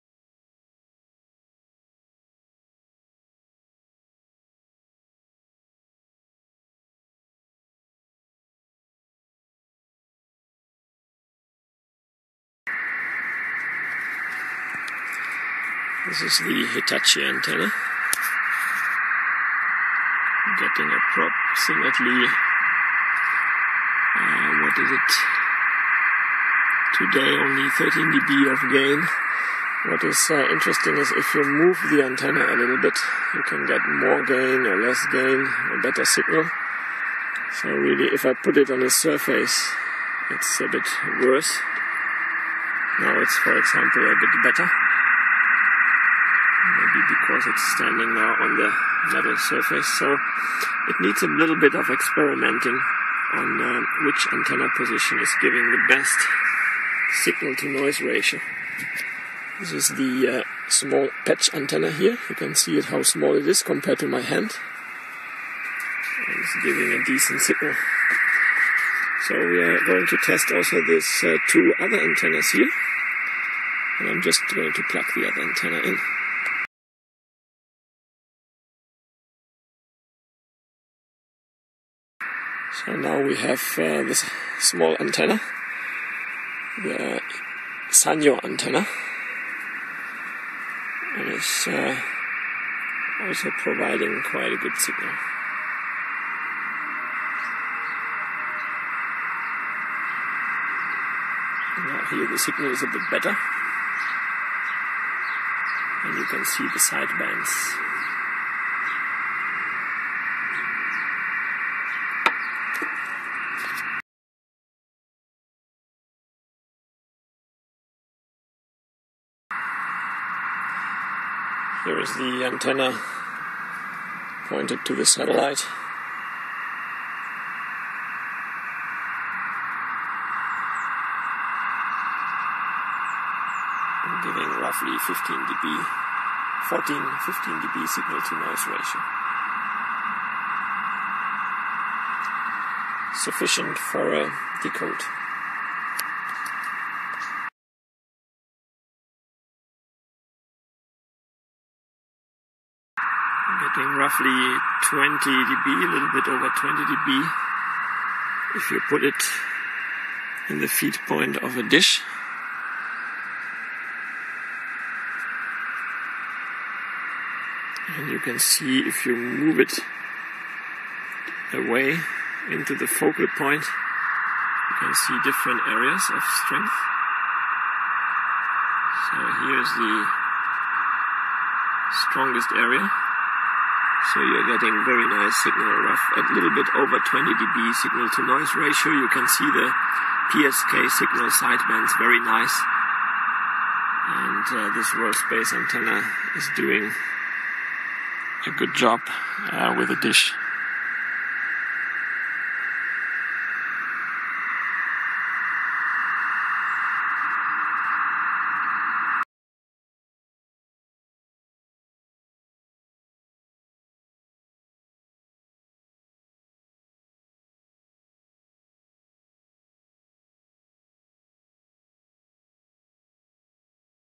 Test: WorldSpace Antenna Comparison on INMARSAT-C Downlink
FOR SALE:   I´ve got a few new and unused WorldSpace Antennas from a stock clearance. I have still 1x Antenna No 2 and 2x Antenna No 3 of this video for sale.

Testing various models of WorldSpace antennas for suitability of receiving"INMARSAT downlink. The original design frequency of this antennas is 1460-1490 MHz, but they seem to work well in the INMARSAT band as well up to GPS frequency band, although gain is usually reduced out of band due to SAW band pass filters at the output of most antenna preamps. Removing the SAW from the preamp and inserting a wire bridge usually yields +10dB additional amplifier gain output on INMARSAT band.